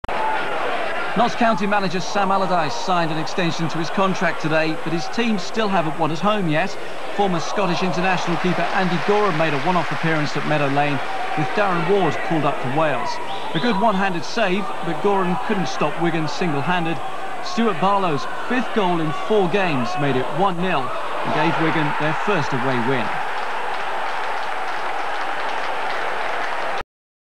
Notts County 0-1 Wigan Athletic - 5th September 1998
Andy Goram in goal for Notts County. Stuart Barlow with the winner.